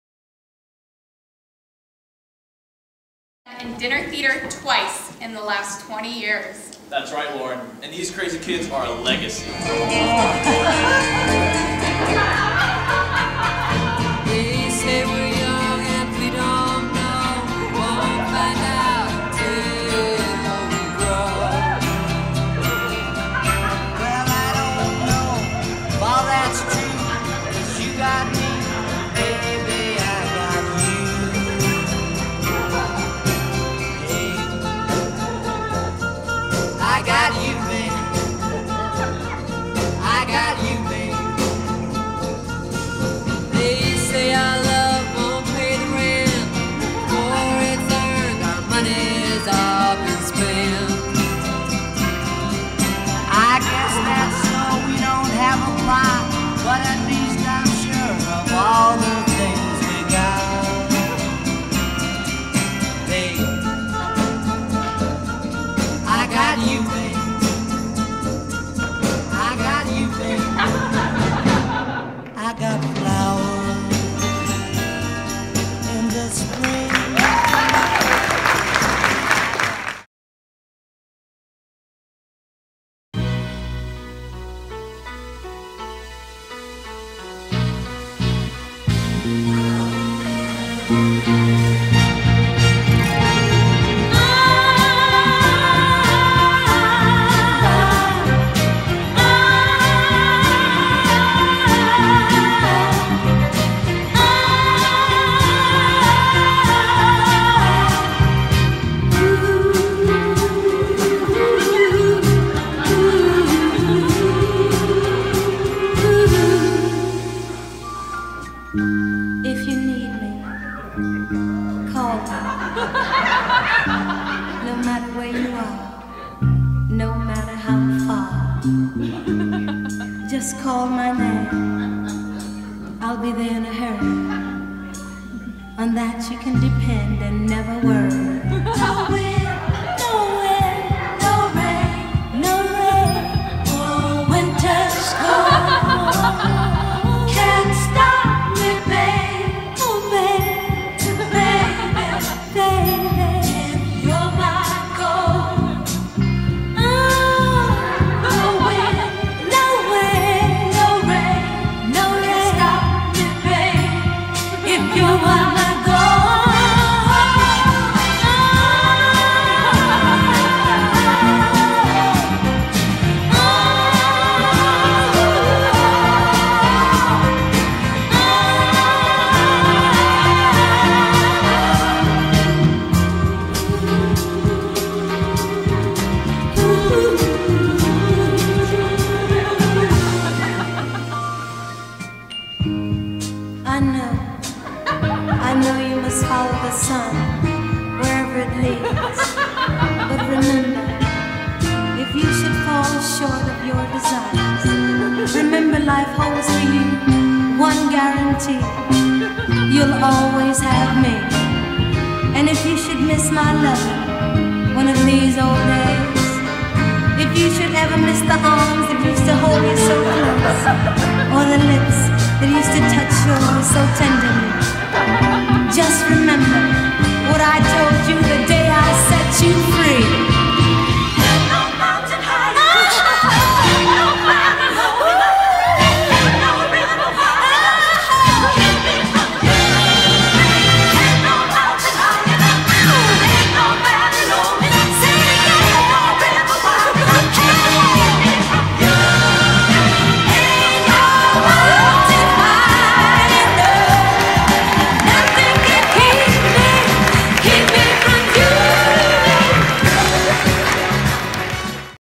PGUMC Youth Group Dinner theater 2016
Justin & Hope (Sonny & Cher); Jacob-Kendall-Harrison (Diana Ross and the Supremes)